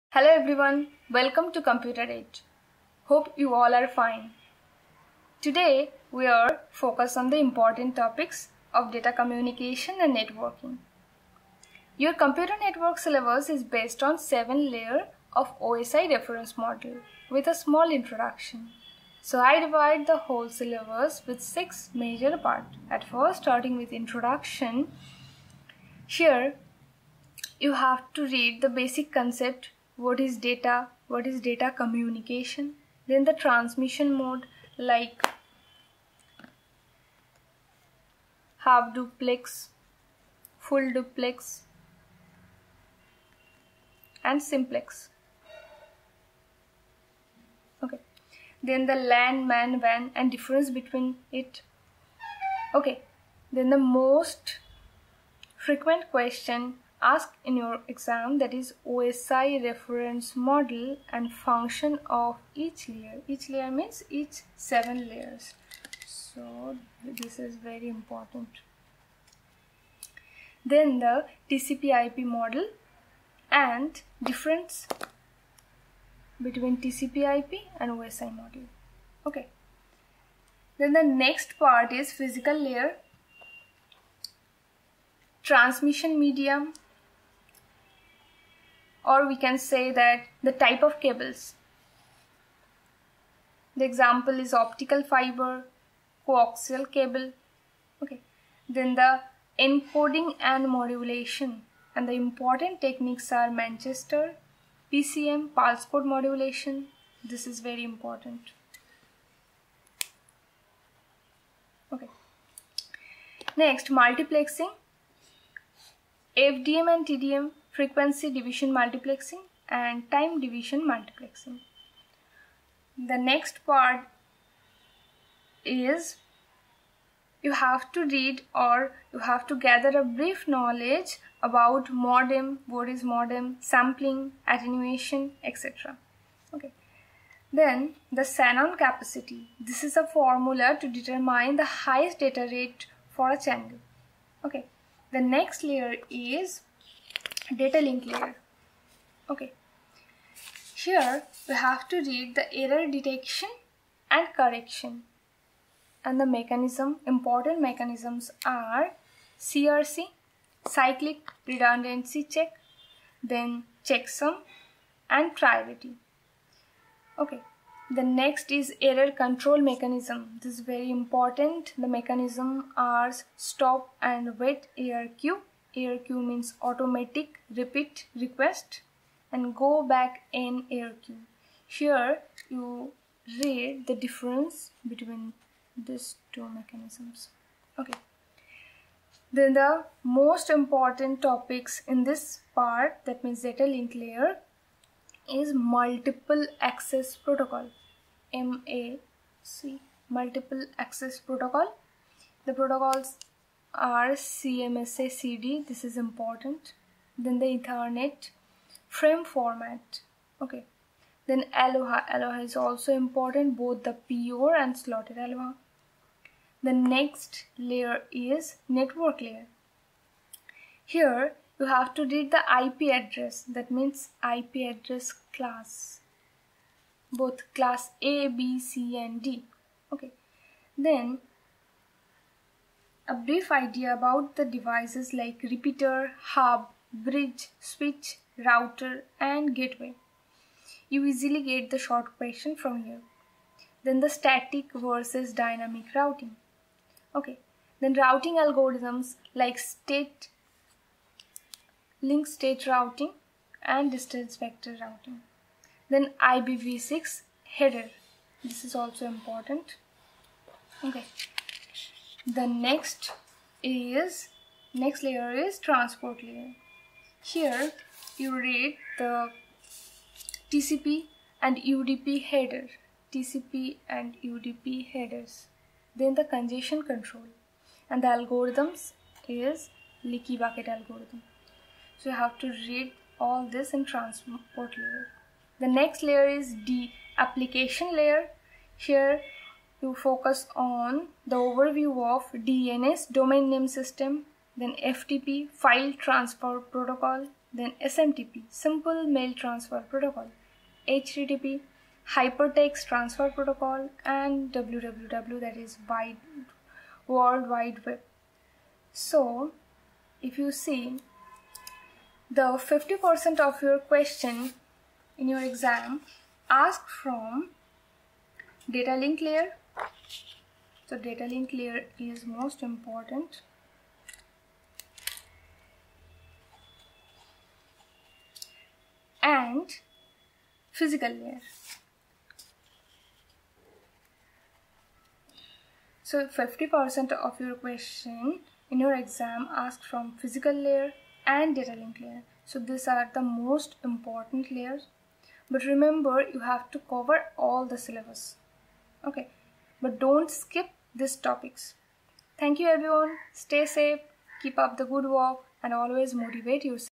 DATA COMMUNICATION AND NETWORKING_ IMPORTANT TOPICS | SYLLABUS
This video help to understand the syllabus and important topics to those student who are studying Networking in this semester.